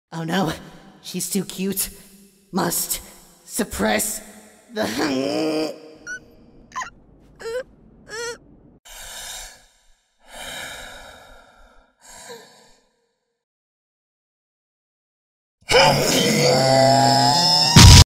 HHHNNNNGGGGGGGGGGG
This is a problem I face regularly, and I'm sure a lot of you do as well.

Audio from: Darling in the FranXX in 10 Minutes by Gigguk (at the 3:33 mark):
(Warning: contains Darling in the FranXX spoilers)